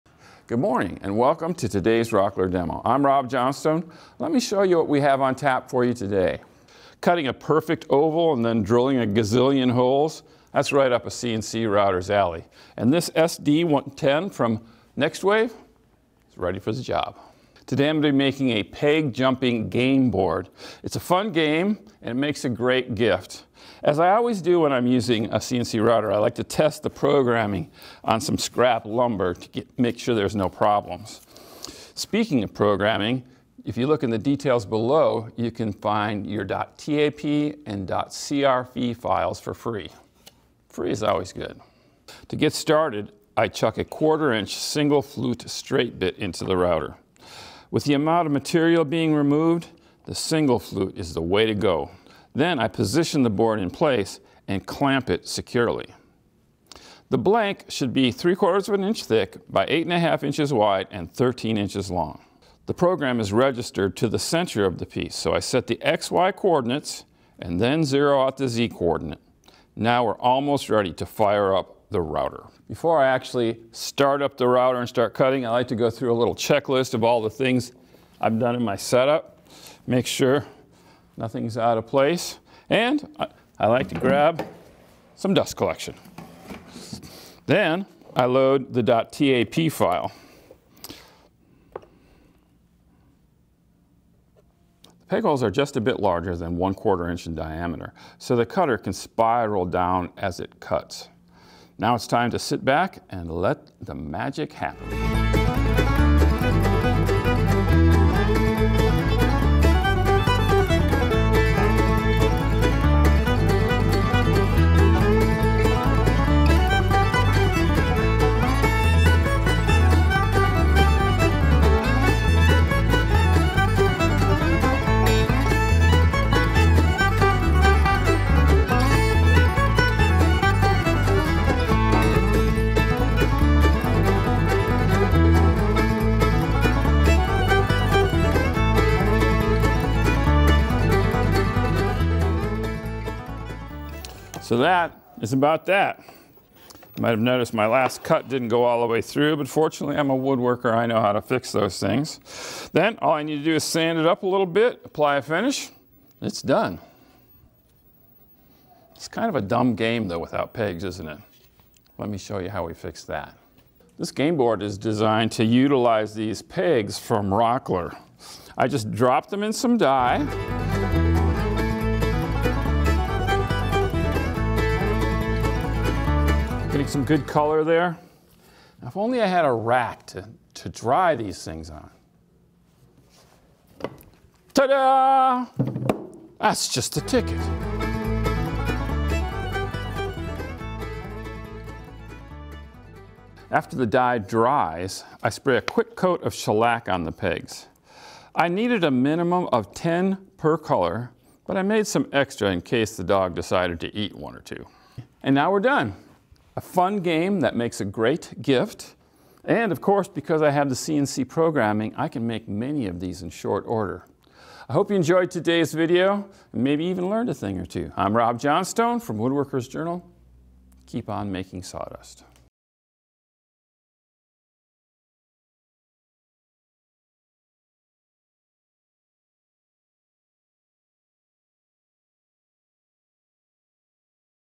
Making a Two-Person Peg Game - CNC Project
Cutting a lot of holes for a game board like this peg jumping game is a perfect job for a CNC machine. Rob Johnstone demonstrates how the NextWave SD110 makes easy work of this task. Then, he shows you how to complete the set by finishing several game pegs, including a practical use for your test board.

Peg Game Rules: This game is a version of Chinese Checkers made for two people.